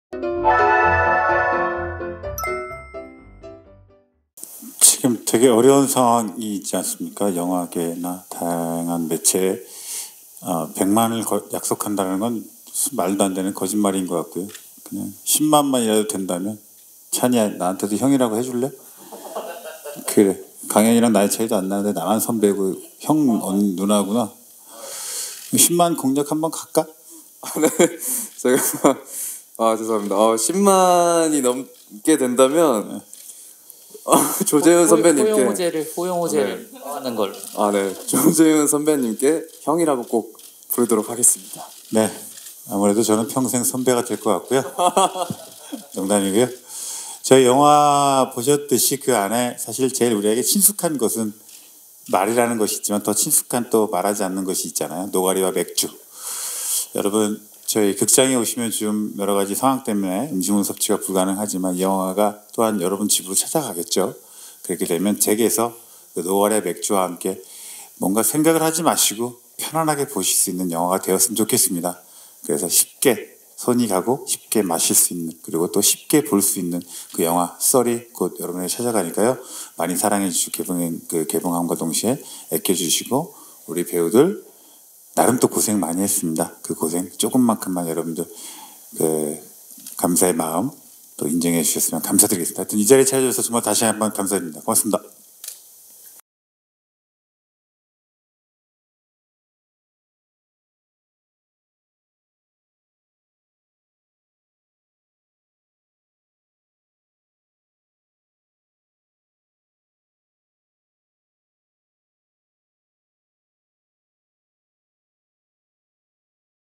"찬희야, 공약 걸어야지?" SF9 강찬희 '썰' 10만 관객 공약 발표!｜ 'CHA NI' A pledge for 100,000 viewers｜무비비
김강현, 강찬희, 김소라, 조재윤 주연의 영화 썰 언론시사회 중
조재현의 압력(?)에 강찬희가 깜짝 공약을 발표했다.